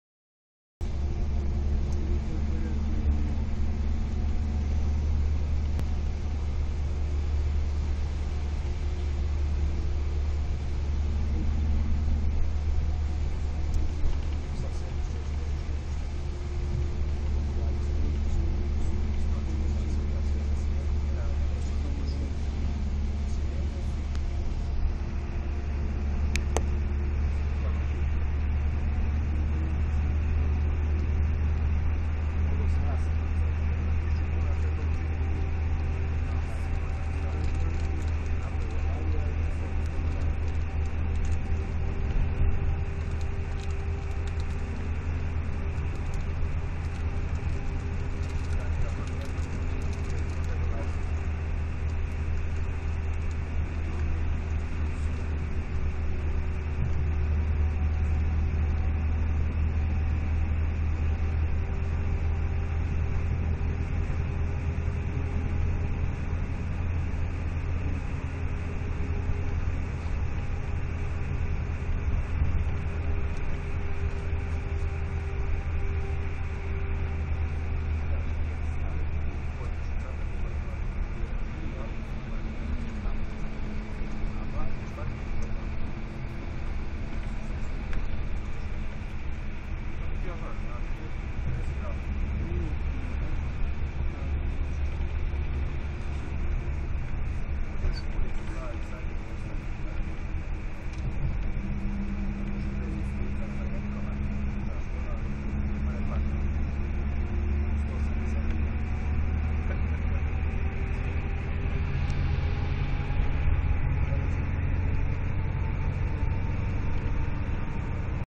između P. Ponikava 082539
driving between Putnikovići and Ponikve

između Putnikoivića i Ponikava
2. prosinca 2013